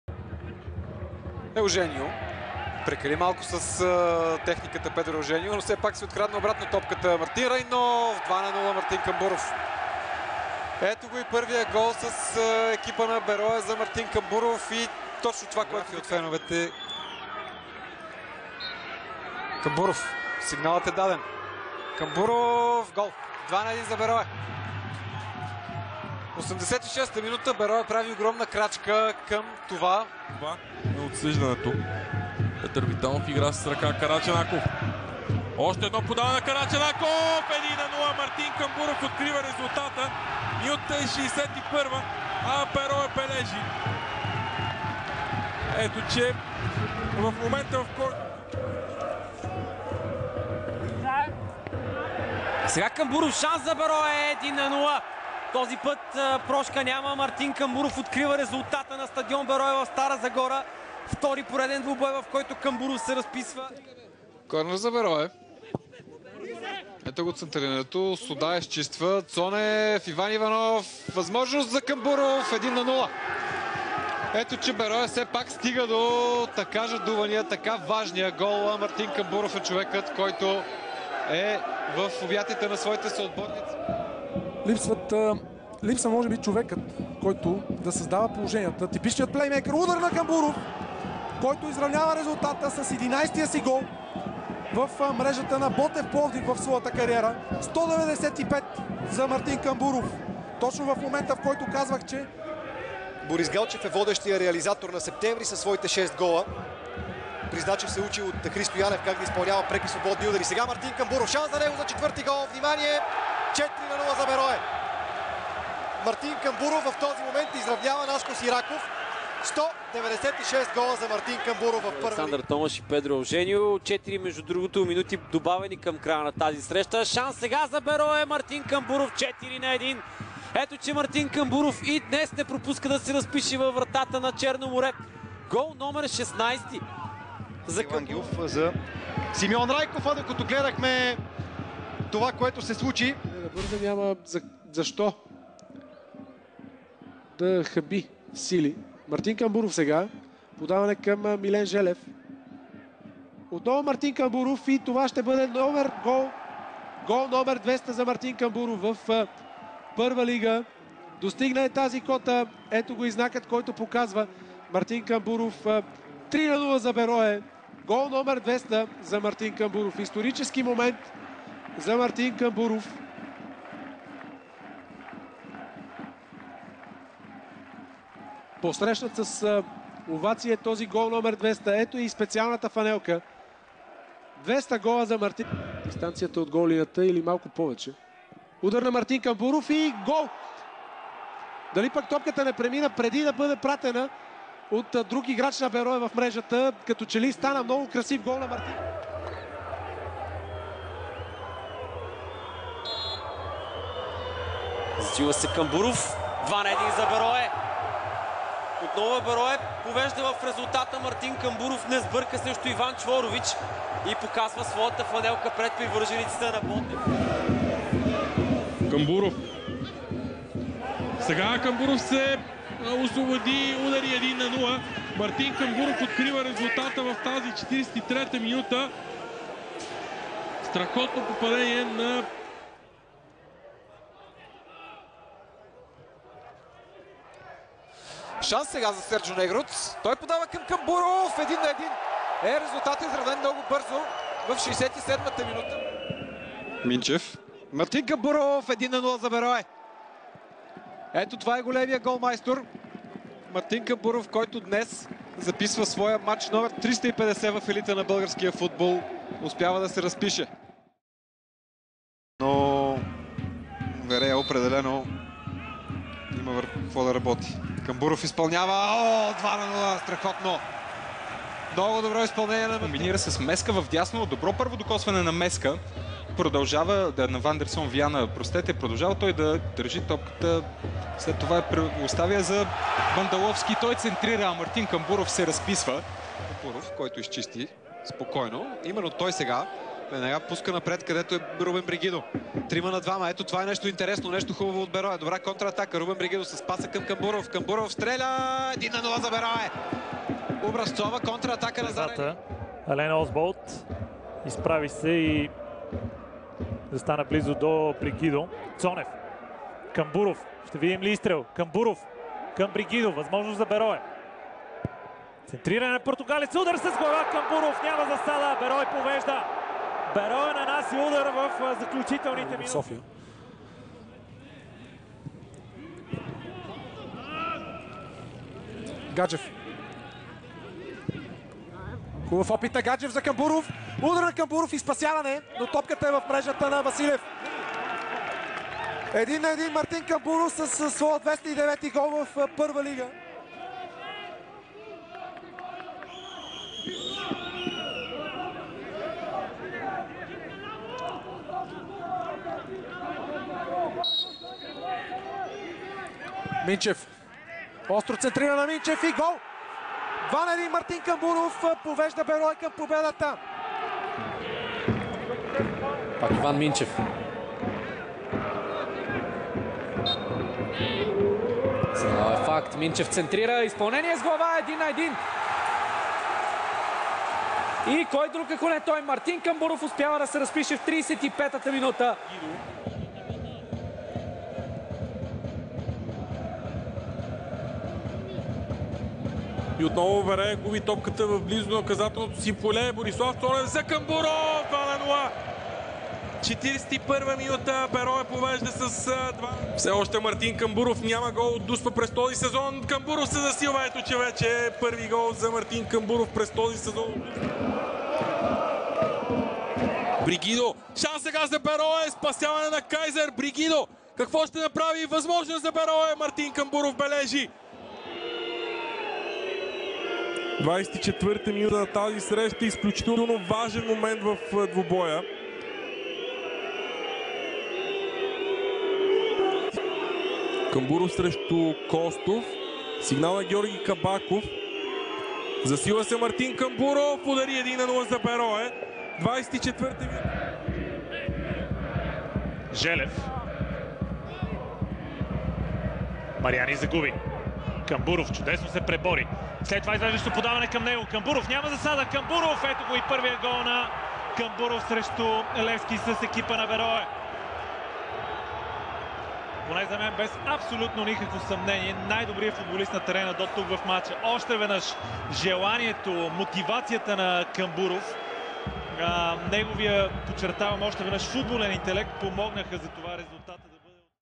Всички голове на Мартин Камбуров за "Берое"
Головете на Камбуров за "Берое" до 1 януари 2019 г.